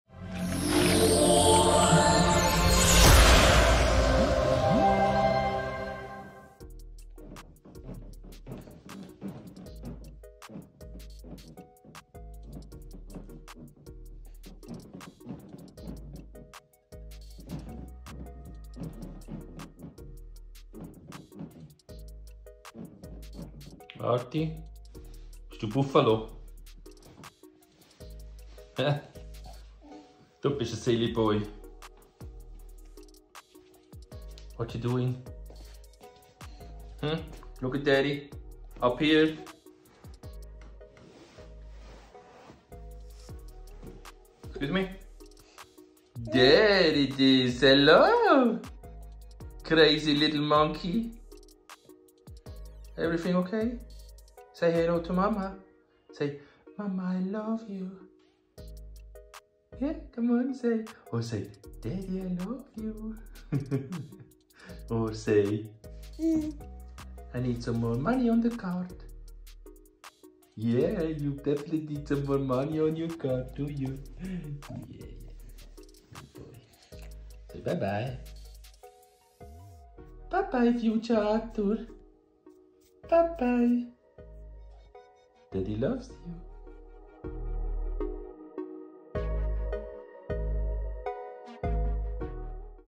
🇮🇪 Baby Boy plays Buffalo - Try not to laugh
Baby Boy plays Buffalo
Baby Boy plays in his bed and Daddy is filming it with his phone.
try not to laugh funny baby
Our family vlog